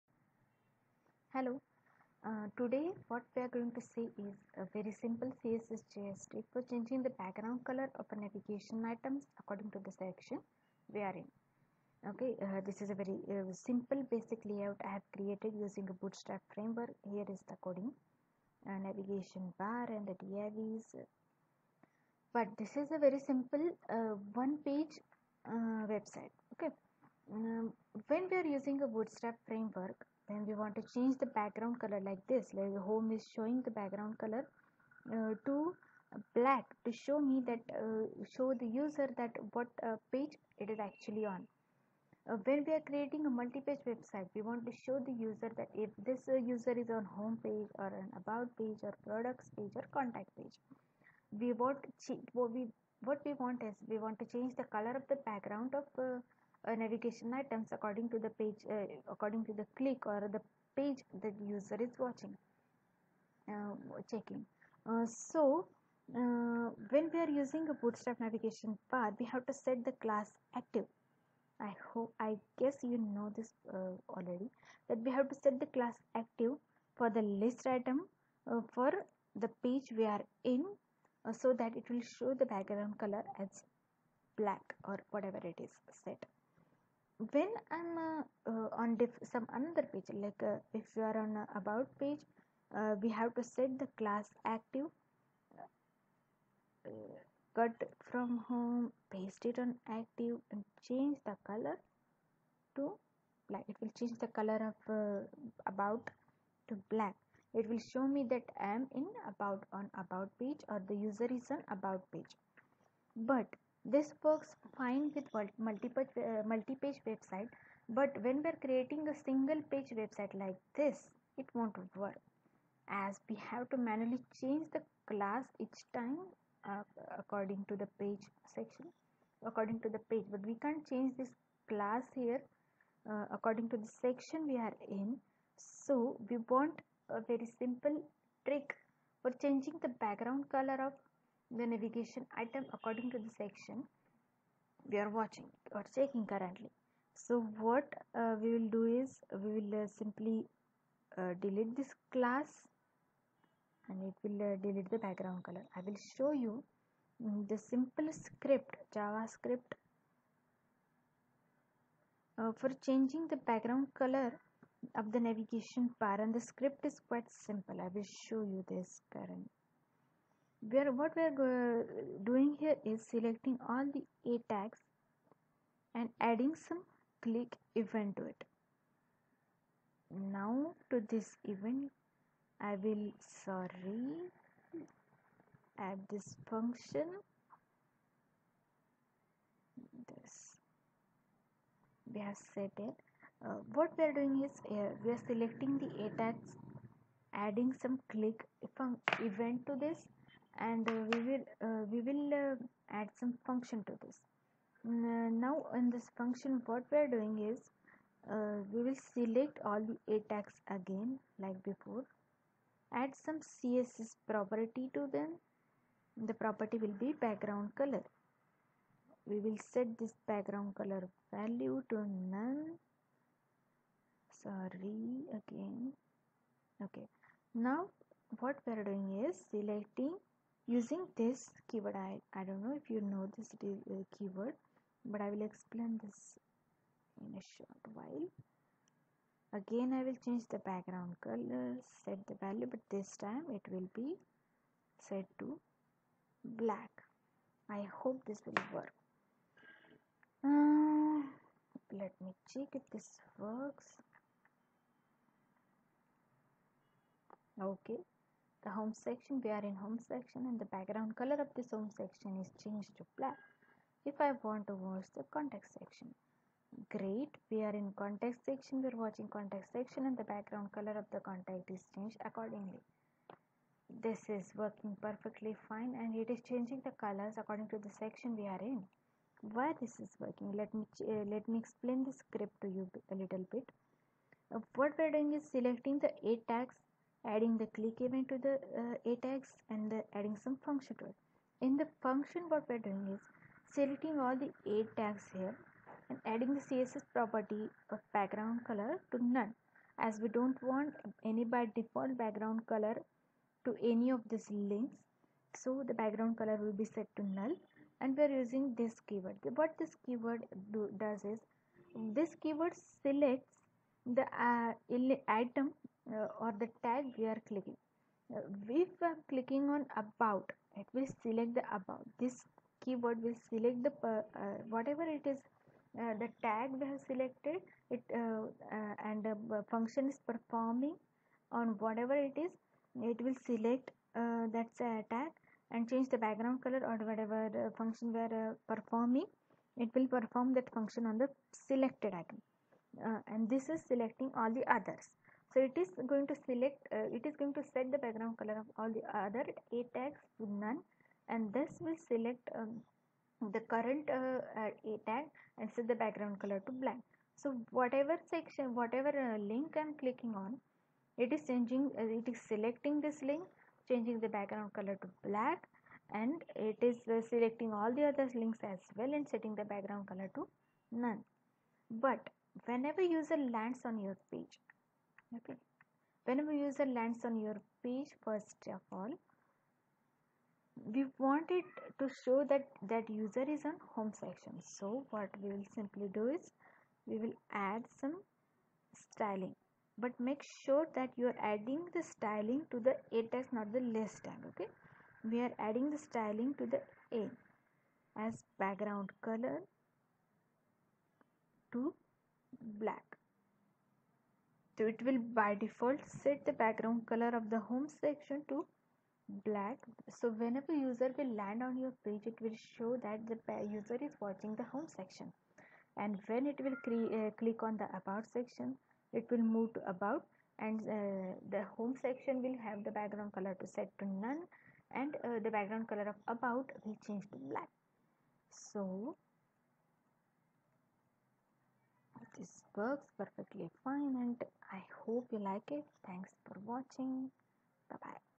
how to change background color of active link navbar
This is very simple css js trick to change background color of active link on navigation bar for one page design.